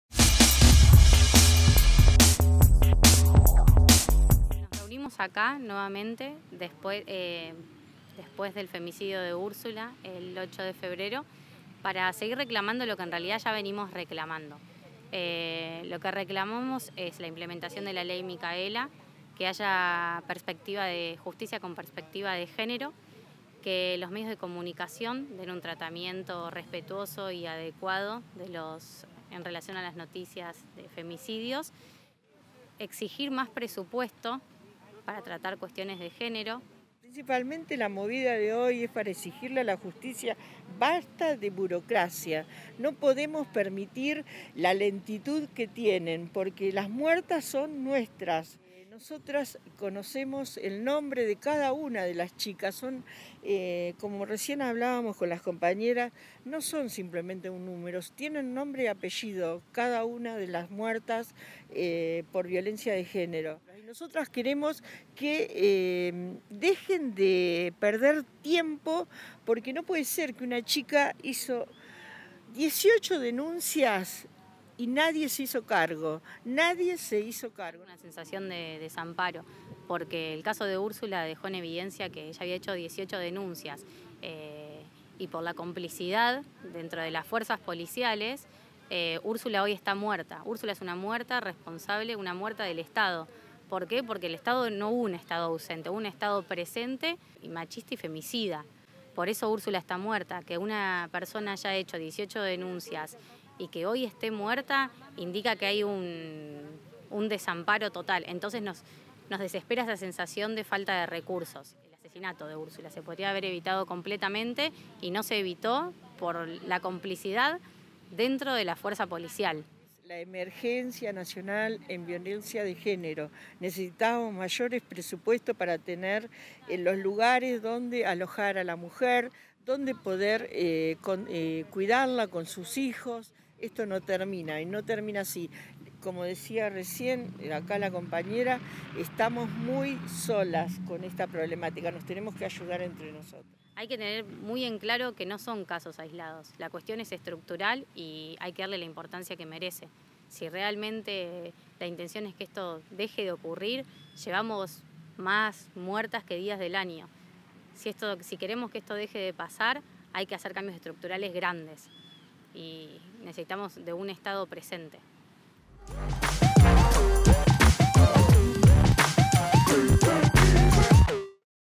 17F - Marcha Nacional
"Por Úrsula y por todes" fue el lema que convocó a nivel nacional a masivas manifestaciones y marchas para pedir justicia ante la elevada cifra de femicidios en lo que va de este 2021.
Las calles de Juárez fueron intervenidas reclamando justicia con perspectiva de género, aceleración en el tratamiento de las causas, implementación de políticas públicas efectivas, y aumento de presupuesto destinado a la lucha contra la violencia de género, entre otros pedidos.
Hablamos con Sonia Campos y Angelina Mangi, parte de las mujeres autoconvocadas para esta movilización.